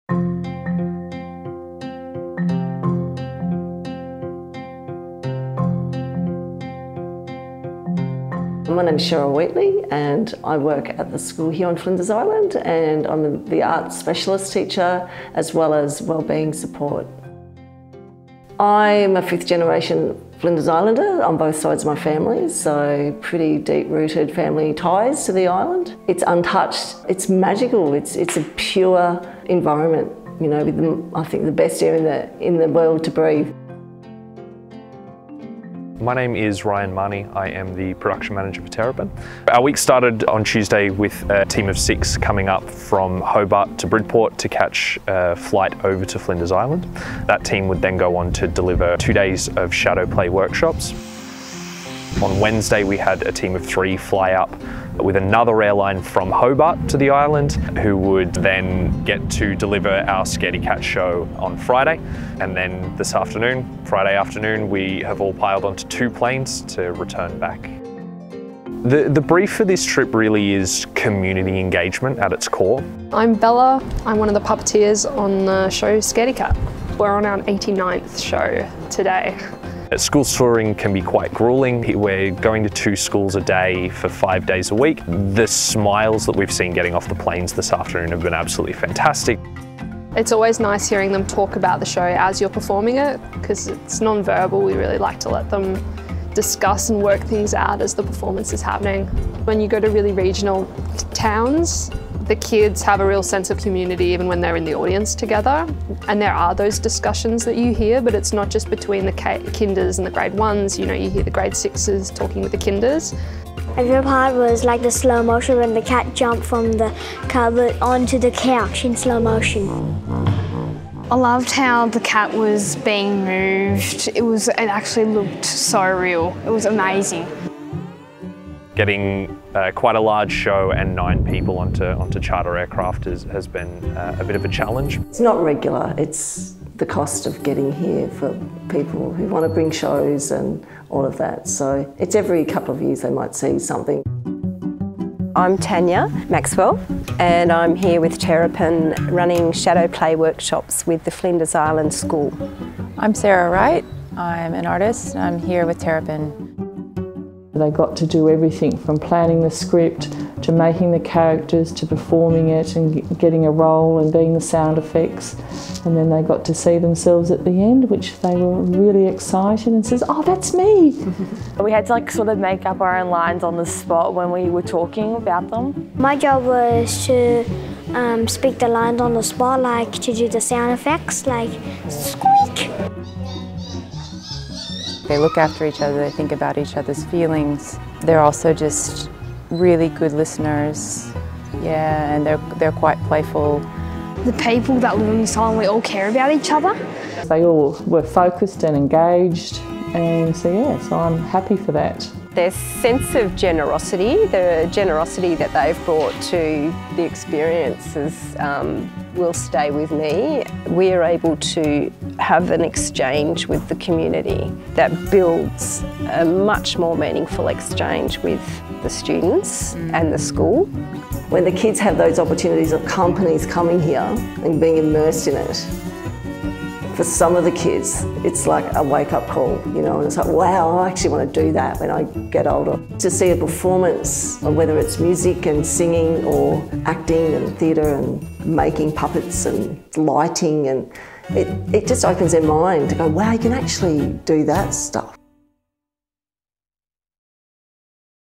A Visit to Flinders Island
In 2021, Terrapin visited remote Flinders Island to present performance Scaredy Cat and Shadow Play workshops.